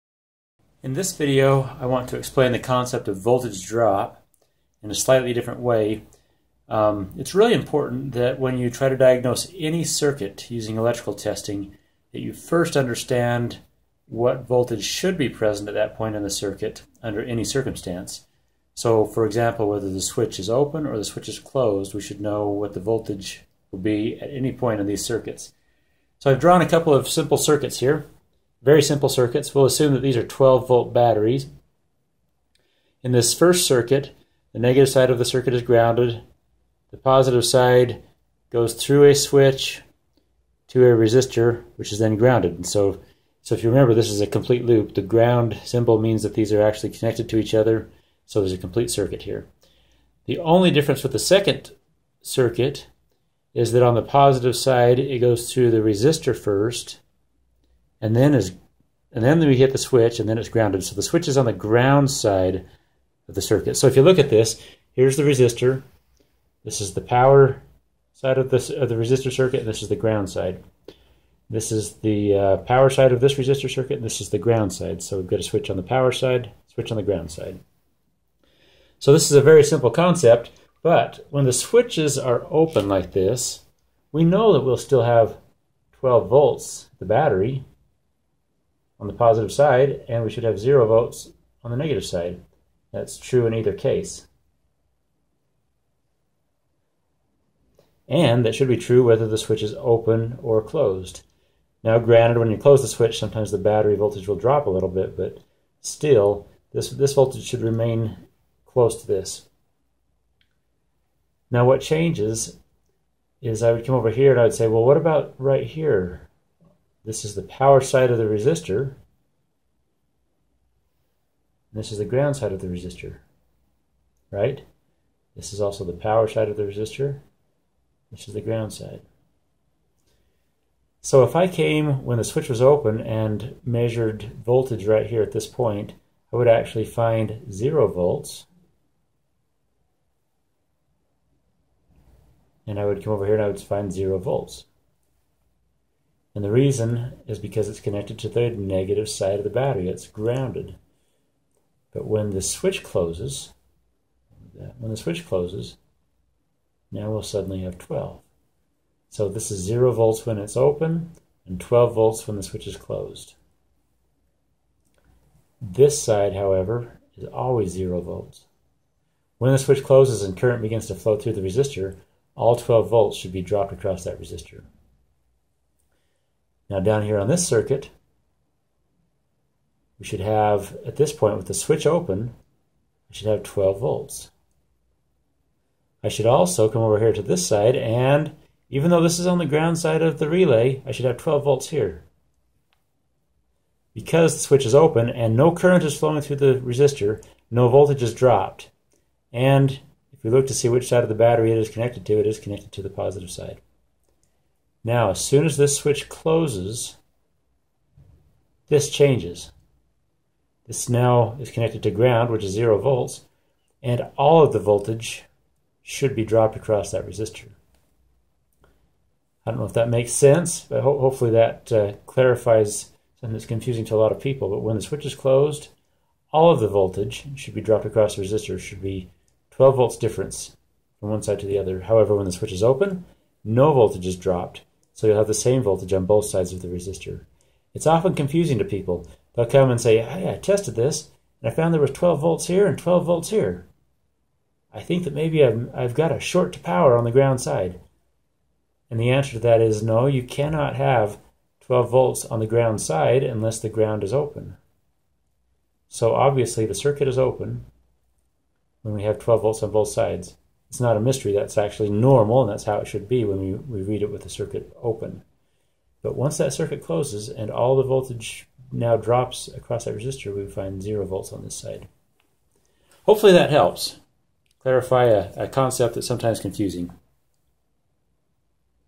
How much voltage should be present at each point in the circuit?
Before you can ever measure voltage in a circuit, you should know exactly what voltage to expect at each point and under each circumstance.  Once you know this, you can test and diagnose easily!